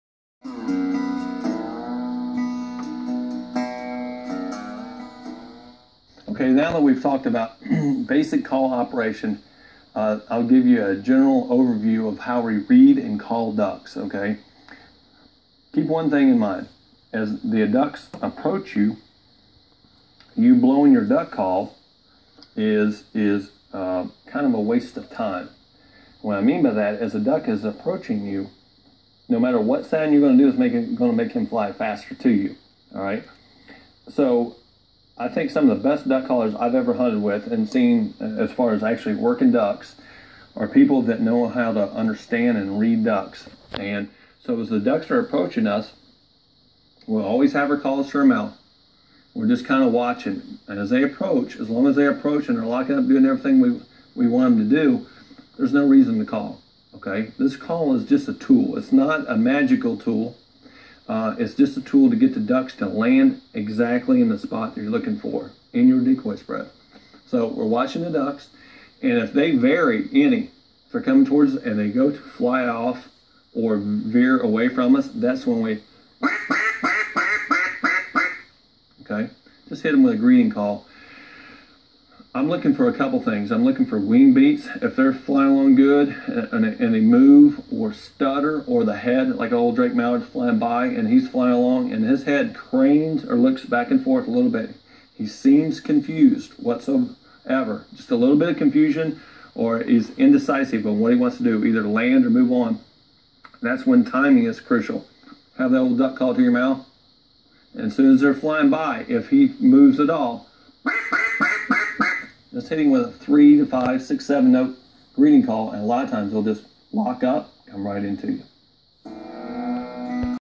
How to read ducks, duck calling, Fred Zink, Zink duck calls,mallard calls,hen and drake,
Fred Zink talks about how to read ducks.

How to read ducks, duck calling, Fred Zink, Zink duck calls,mallard calls